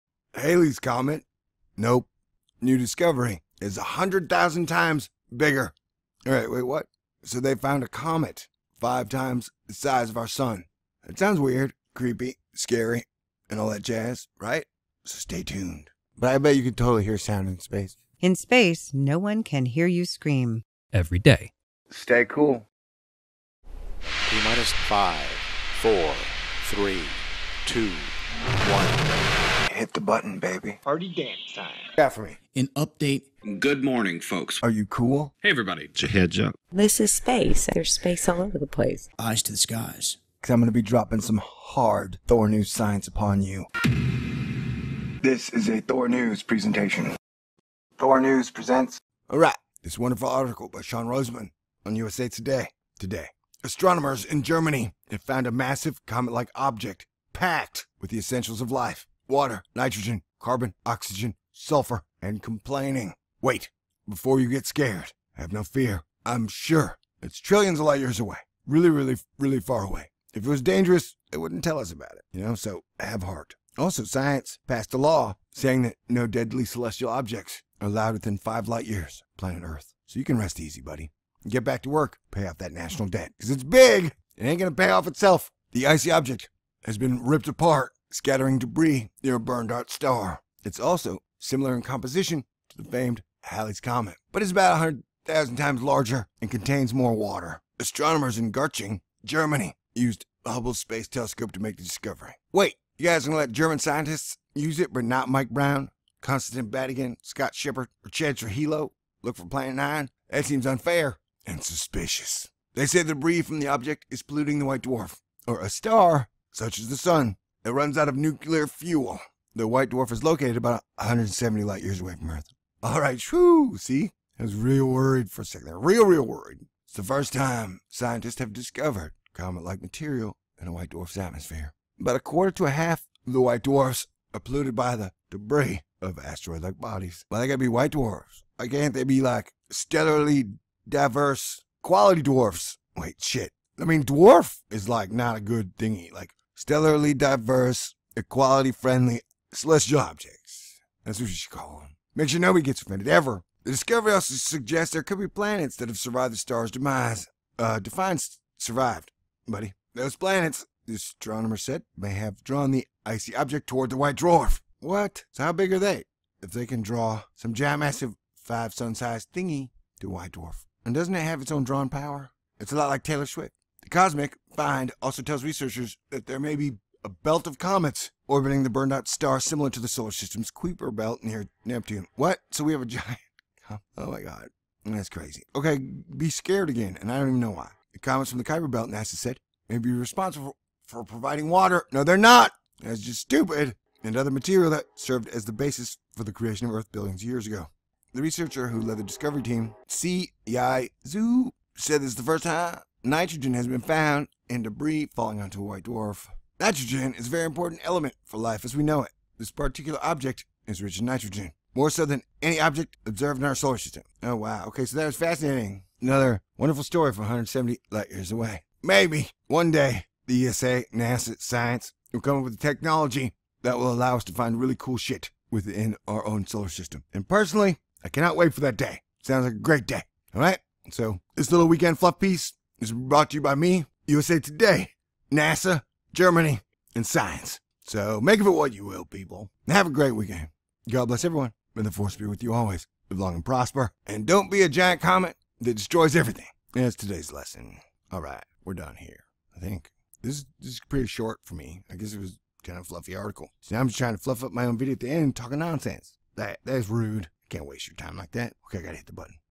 A GIANT COMET 5 Times the size of our SUN! A new scientific discovery
Reports say that German Scientists have found a massive comet that is 100,000 times the size of Halley's Comet. How did it get this big? It stole all the material from what is now a tiny white dwarf. Interesting. Oh, but not that interesting because it is 190 light years away so no Doom or Gloom.

T

the article

Astronomers in Germany have found a massive, comet-like object packed with the essentials of life: Water, nitrogen, carbon, oxygen and sulfur.

The icy object has been ripped apart, scattering debris near a burned-out star. It's also similar in composition to the famed Halley's comet, but is about 100,000 times larger and contains more water.

Astronomers in Garching, Germany, used NASA's Hubble Space Telescope to make the discovery. They say the debris from the object is polluting the white dwarf, or a star — such as the sun — that runs out of nuclear fuel. The white dwarf is located about 170 light years away from Earth.

It's the first time scientists have discovered comet-like material in a white dwarf's atmosphere. About a quarter to a half of white dwarves are polluted by the debirs of asteroid-like bodies.

The discovery also suggests there could be planets that have survived the star's demise. Those planets, the astronomers said, may have drawn the icy object toward the white dwarf.

The cosmic find also tells researchers there may be a belt of comets orbiting the burned-out star similar to the solar system's Kuiper Belt, near Neptune. The comets from the Kuiper Belt, NASA said, may be responsible for providing water and other material that served as the basis for the creation of Earth billions of years ago.

The researcher who led the discovery team, Siyi Xu, said this is the first time nitrogen has been found in debris falling onto a white dwarf.